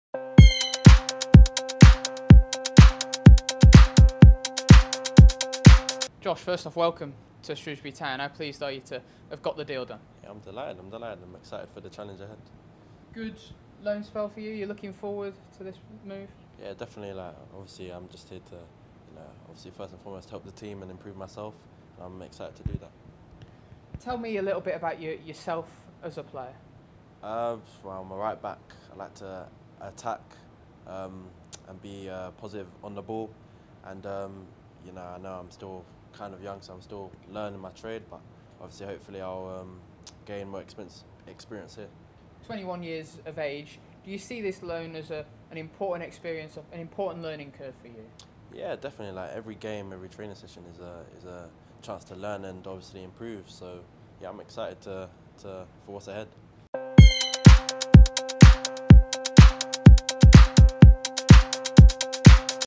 INTERVIEW | Josh Emmanuel on joining Town - Town TV
Defender Josh Emmanuel reacts to joining Shrewsbury Town on a season-long loan deal from Ipswich Town - full interview available on IFollowShrews